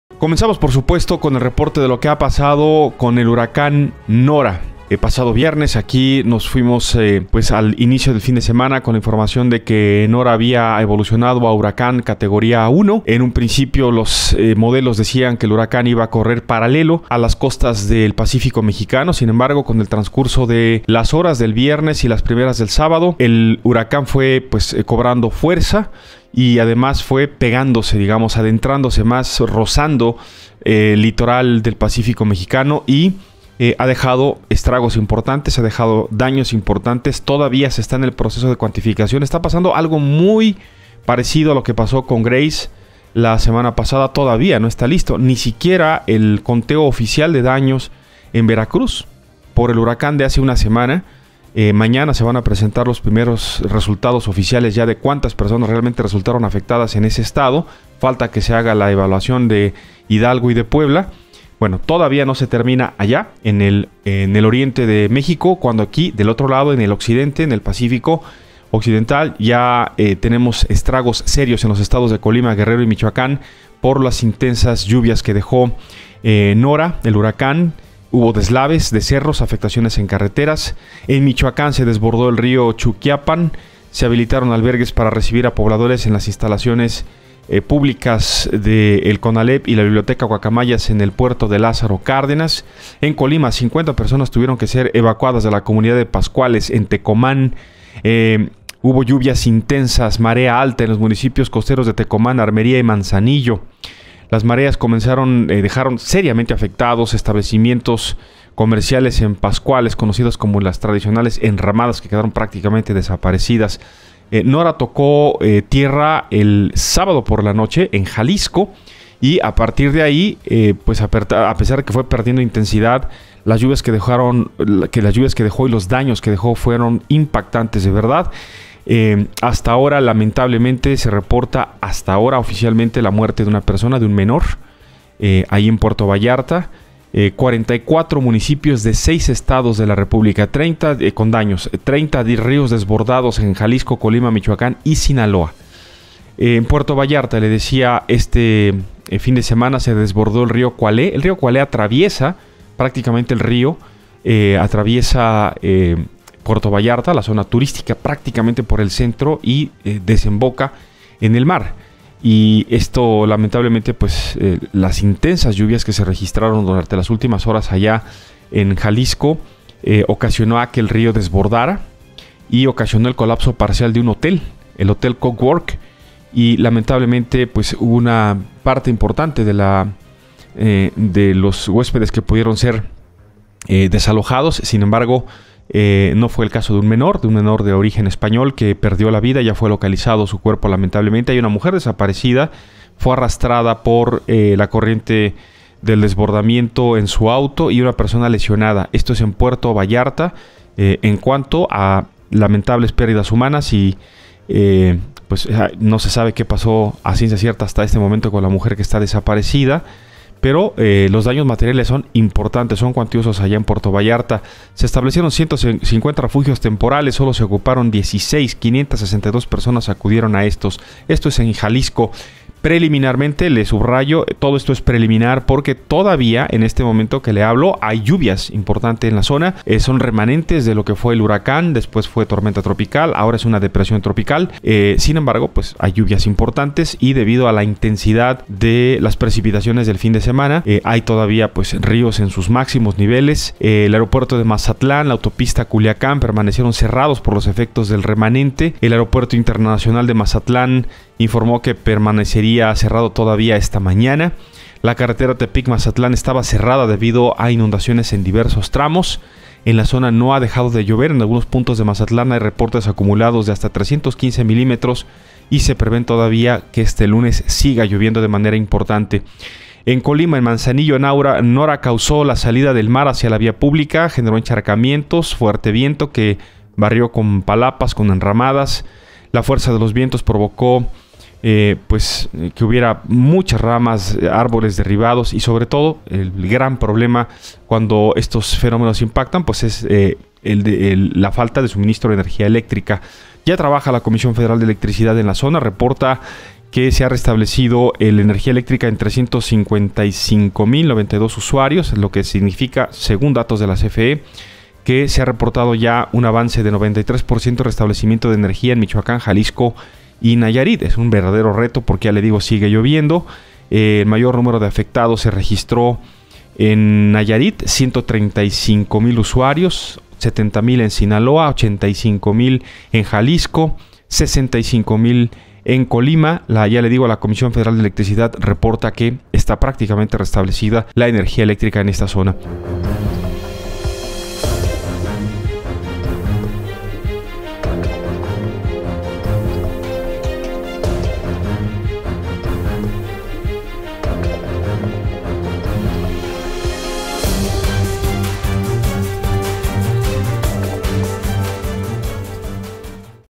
Nora azotó a Michoacán, Colima, Jalisco y Nayarit, se evalúan daños
El huracán Nora dejó por saldo una persona muerta, daños en 44 municipios de seis estados, 30 ríos desbordados en Jalisco, Colima, Michoacán y Sinaloa y varios árboles caídos.